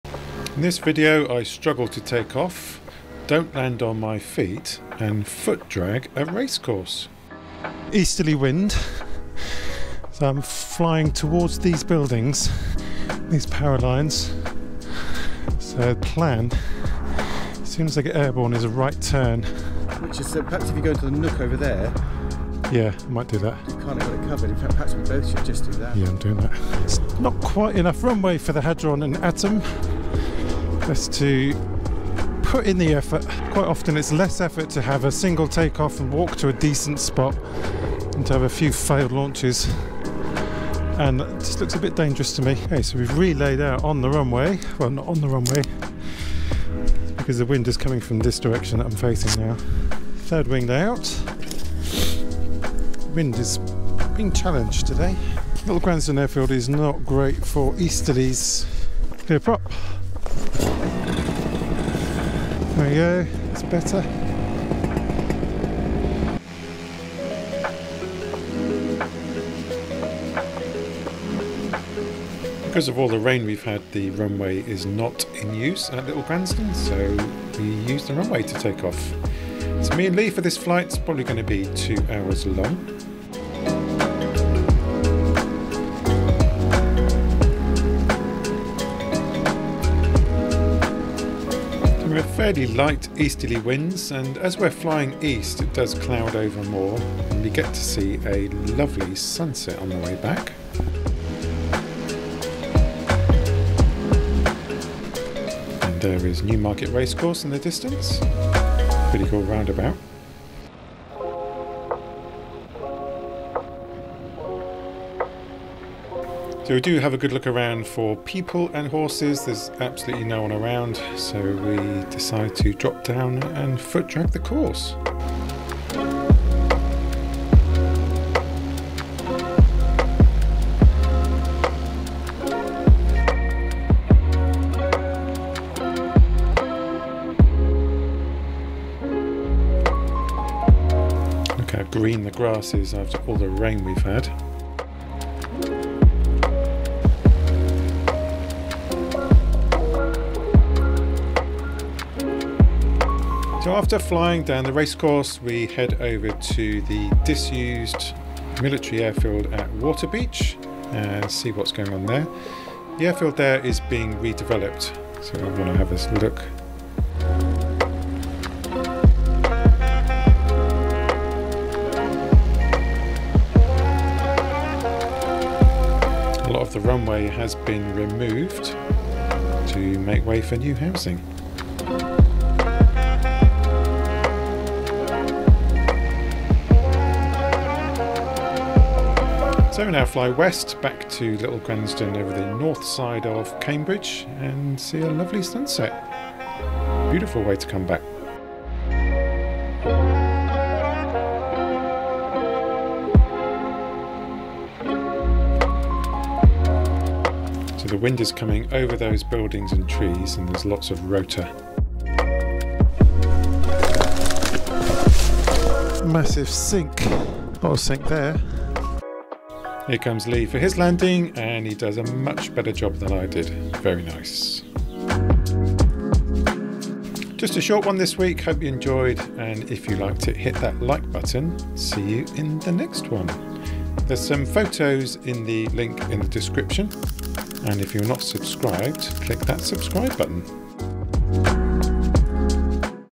I Got Dumped in Rotor
We fly low over a racecourse but struggle to take off and land.

🛠️ My Gear
Wing: Dudek Hadron 3 20m
Motor: Bulldog Spirit Atom 80 MY22
Helmet Camera
Photo camera
Video editor: Davinci Resolve 18.6